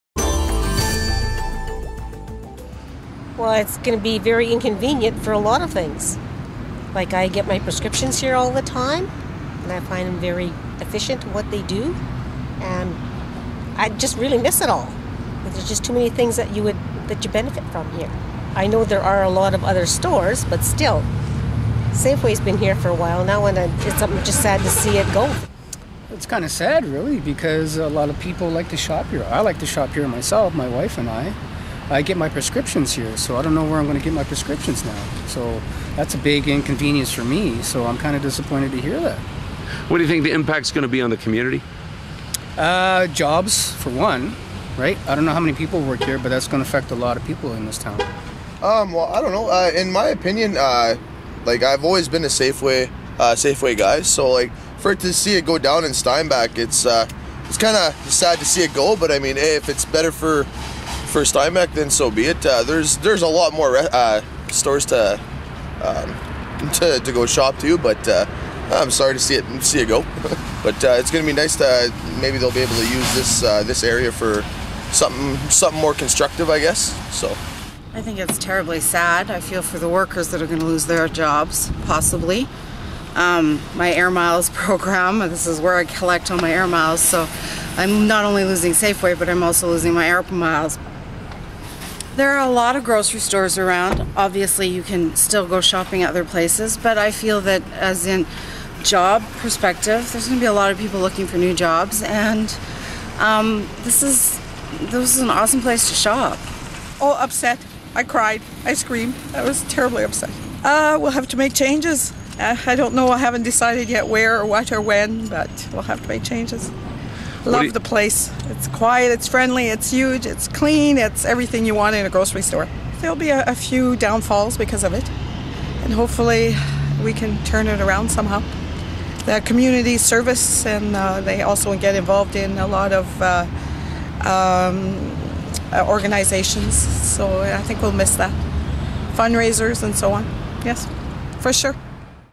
Customers React To Safeway Closing
Steinbach Safeway location will be closing down. Here's what their customers think.